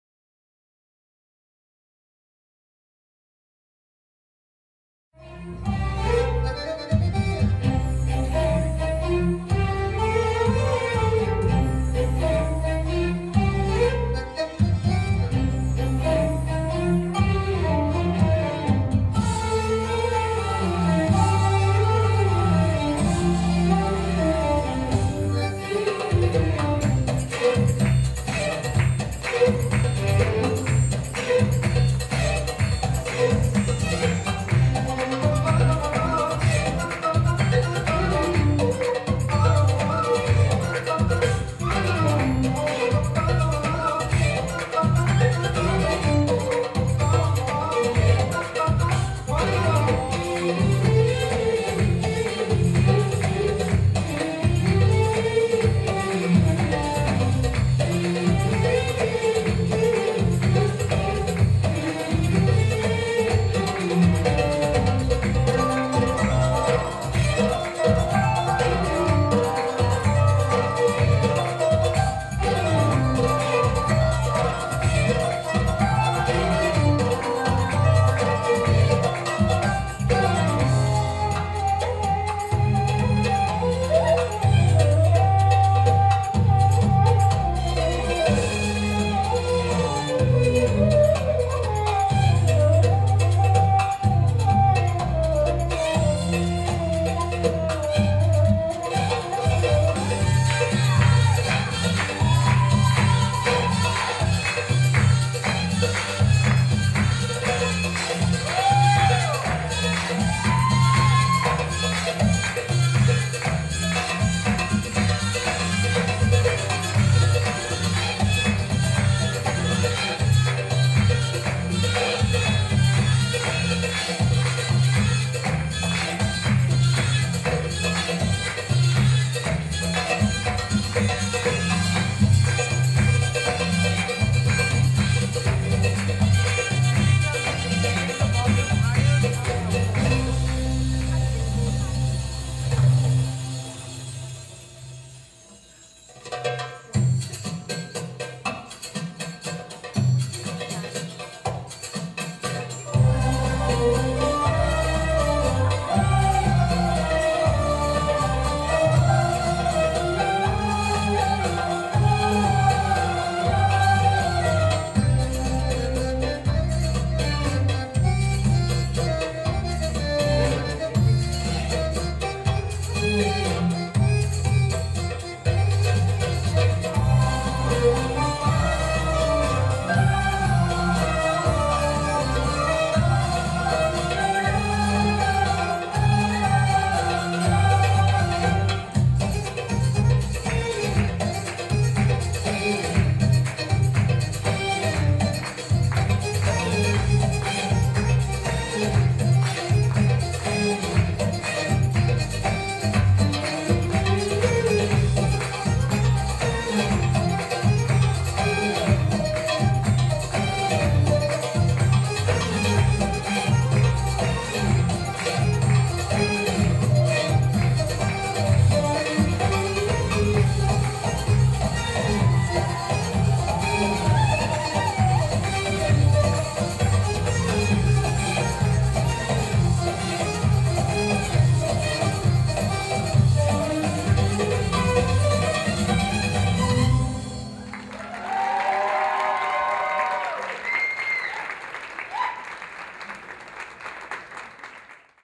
Alexandra Molina at Houston Oriental and Folklore Dance Festival 2015
Alexandra of Belly Motions Miami performance at Houston Oriental and Folklore Dance Festival 2015.
Video by BK Photography.